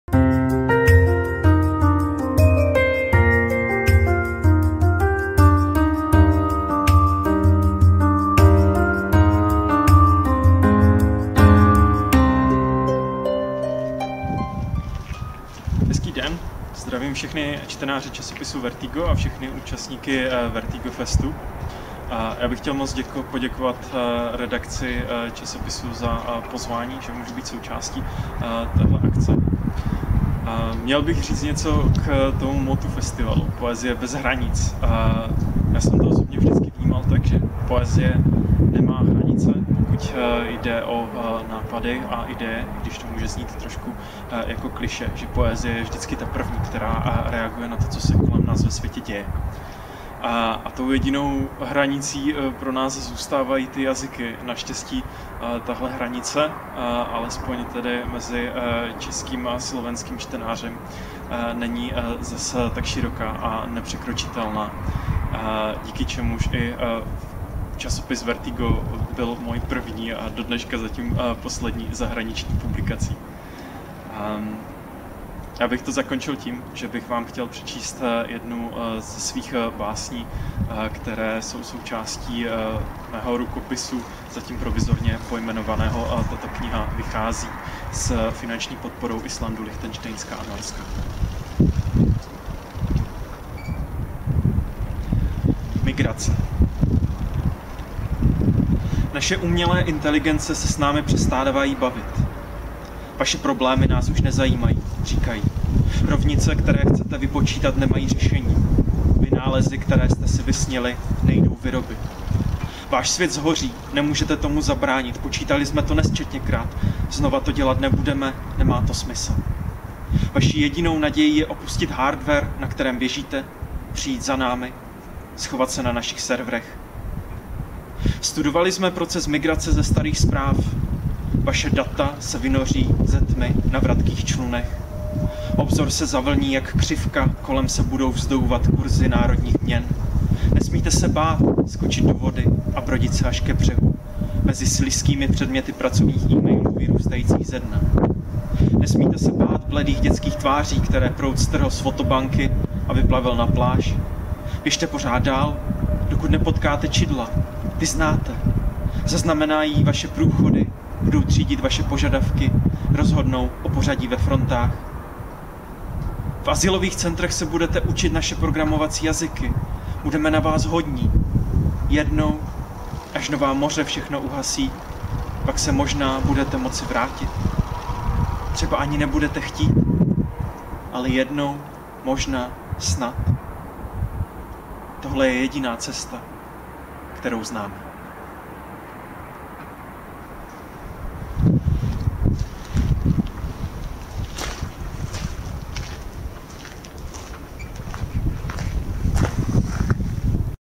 Jan Nemček [CZ] - Vertigo fest 2021
EN: Jan Nemček is a Czech poet, debuted with poetry collection Proluka (Perplex, 2016) he was nominated for the Jiří Orten Prize for these book. He was nominated for the Dresden Lyric Prize in 2018. Now he works on new poems. He lives in Ostrava.

SK: Jan Nemček je český básnik, debutoval básnickou zbierkou Proluka (Perplex, 2016), za ktorú bol nominovaný na Cenu Jiřího Ortena. V roku 2018 bol nominovaný na Drážďanskú cenu lyriky. Aktuálne pracuje na novej zbierke. Žije v Ostrave.